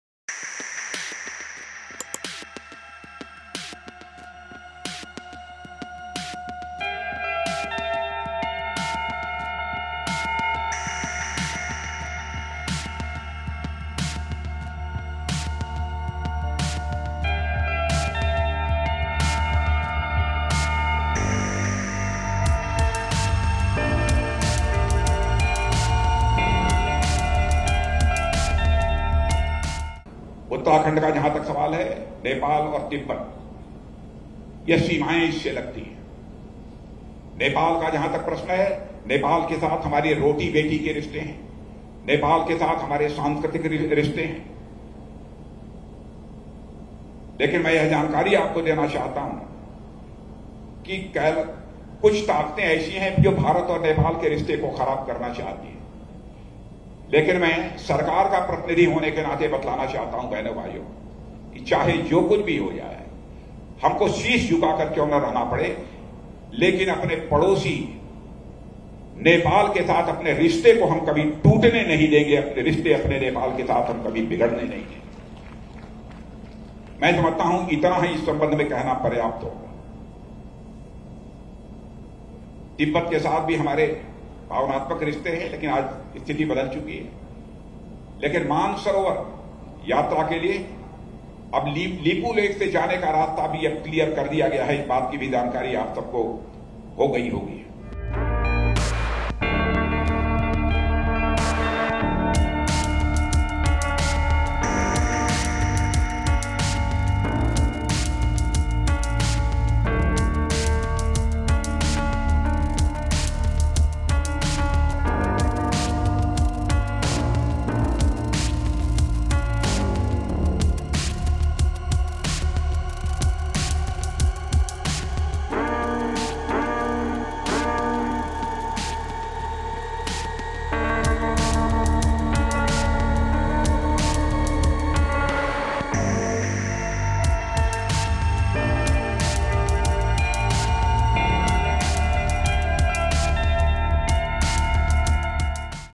'Will not let...': Watch how Rajnath Singh took a veiled swipe at China on India-Nepal ties
Union Defence Minister Rajnath Singh takes a veiled swipe at China. Rajnath Singh said we will not let ‘some forces’ spoil India’s relations with Nepal. China is increasingly entrenching itself in Nepal with a slew of long-term agreements.